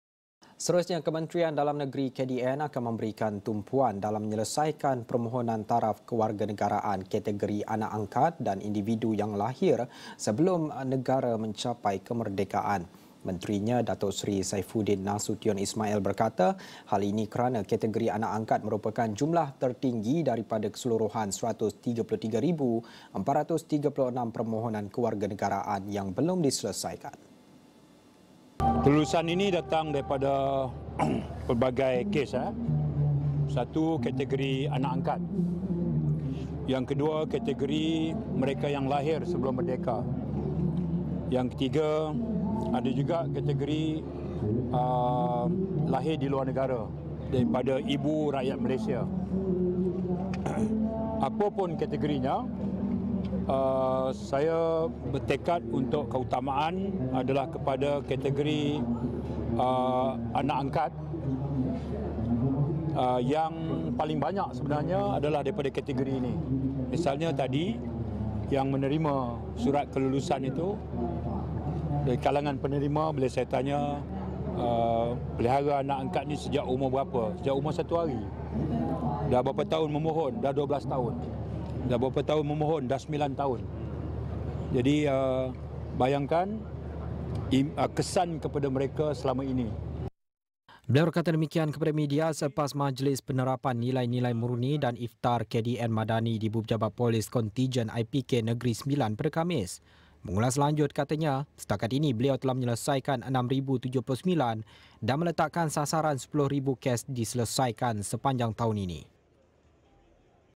KDN tumpu selesai isu anak angkat
Kementerian Dalam Negeri (KDN) akan memberi tumpuan dalam menyelesaikan permohonan taraf kewarganegaraan kategori anak angkat dan individu yang lahir sebelum negara mencapai kemerdekaan.

Menterinya, Datuk Seri Saifuddin Nasution Ismail berkata, hal ini kerana kategori anak angkat merupakan jumlah tertinggi daripada keseluruhan 133,436 permohonan kewarganegaraan yang belum diselesaikan.